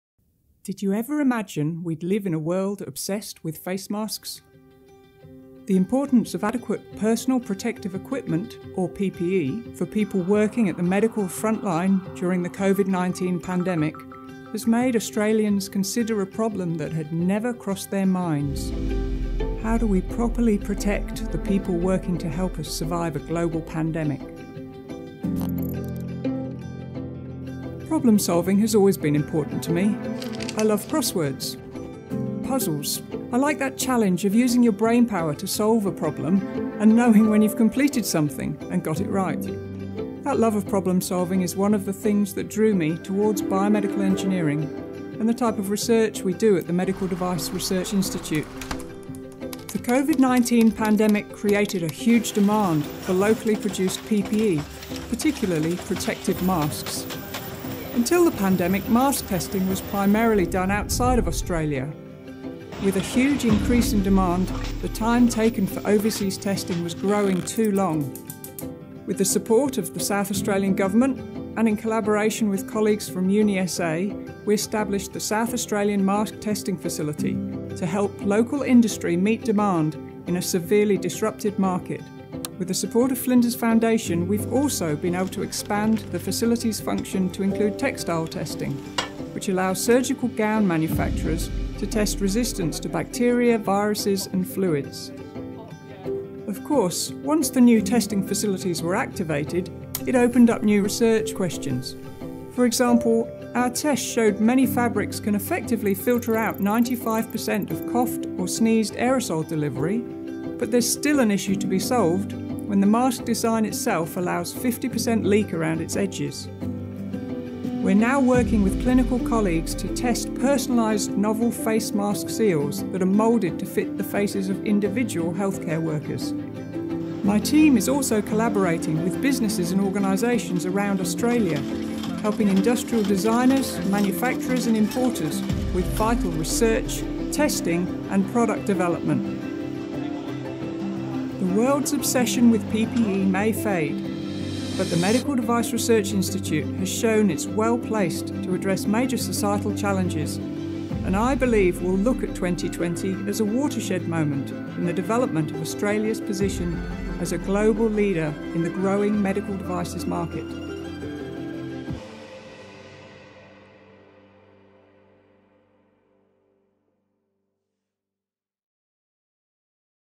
BRAVE MINDS | Strategic Research in Motion
The need was urgent.
COVID-19 triggered enormous demand for adequate personal protective equipment (PPE) for people working at the medical ‘front line’. This challenge set Professor Karen Reynolds’ strategic research brain in motion.

Discover how Flinders is making a difference to our culture, economy, environment society and world.